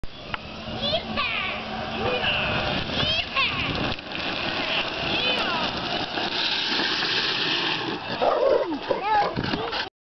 Shotgun Spin Out
Me riding shotgun with JT on a spin out.